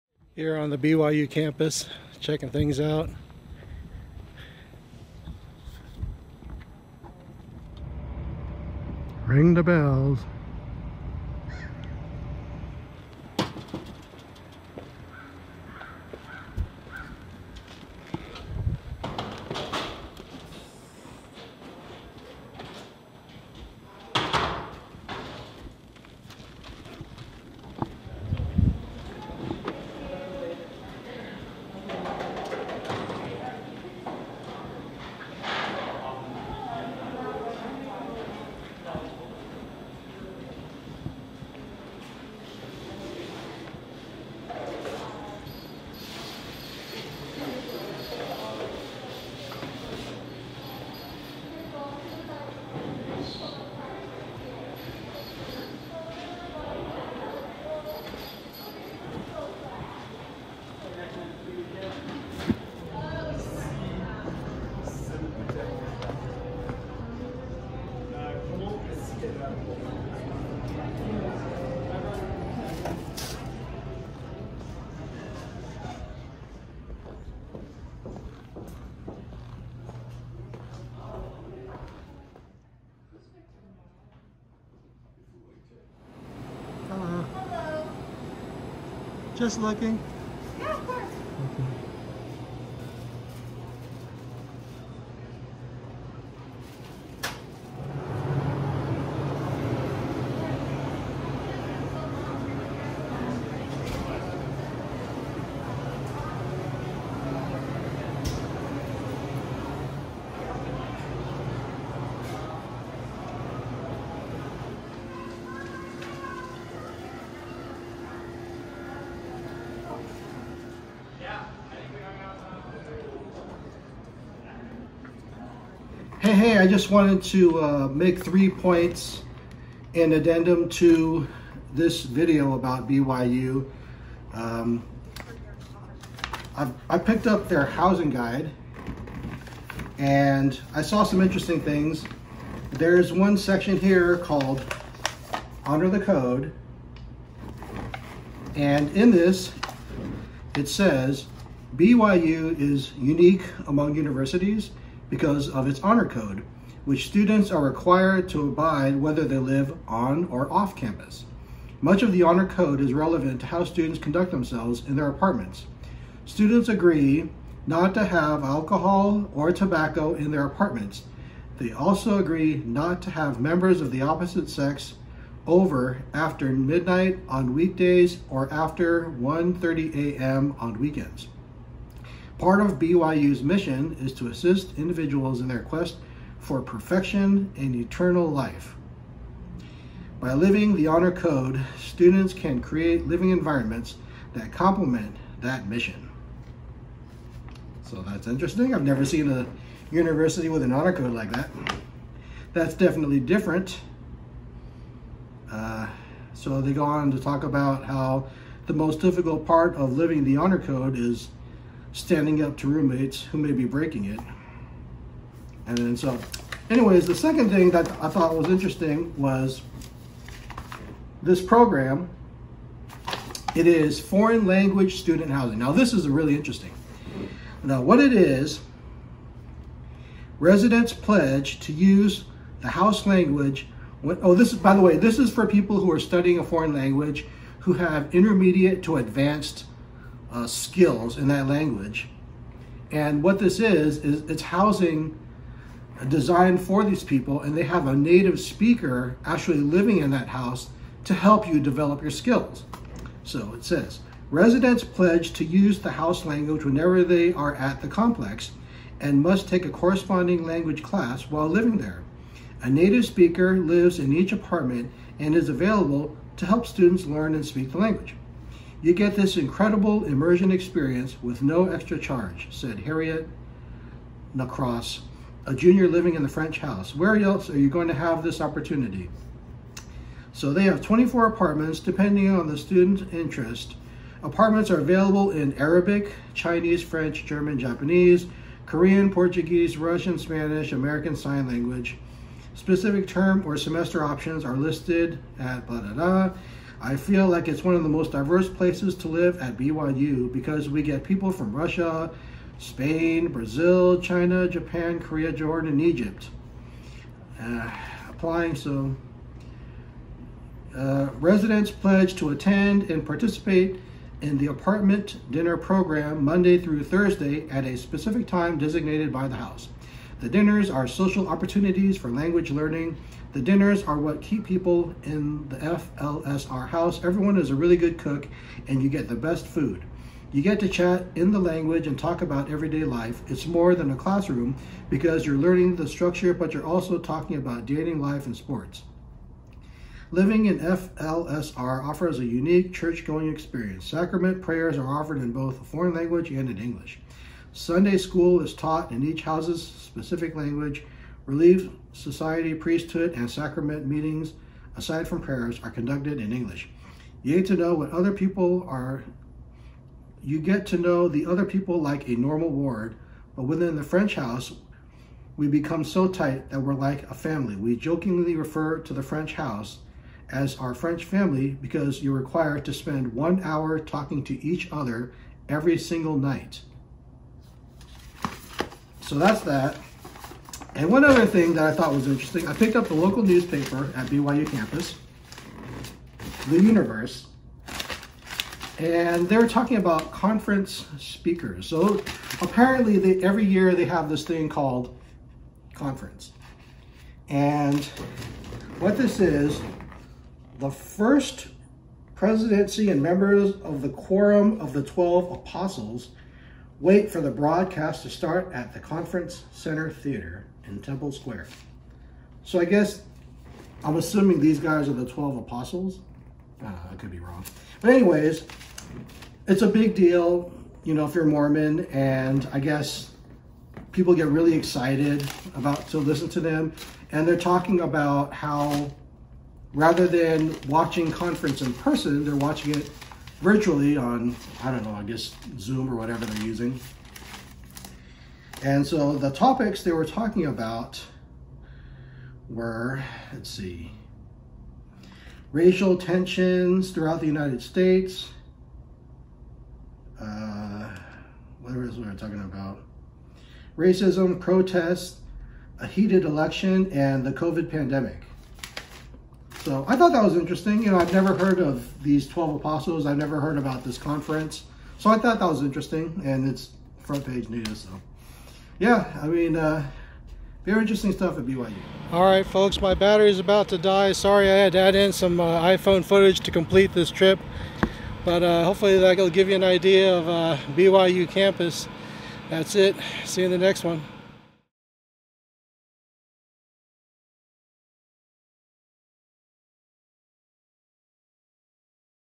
Visiting Brigham Young University (BYU) in Provo Utah!
Today I'm visiting BYU campus in Provo Utah. They have some interesting ways of doing things over there. In this video I share some of the unique ways of life on campus. For this year, there has been a rule change. Mountain Dew may now be purchased on campus. The soft drink was originally banned because it had caffeine in it. Also, in addition to no caffeine or alcohol, the Code of Honor also includes no swearing, which I didn't mention.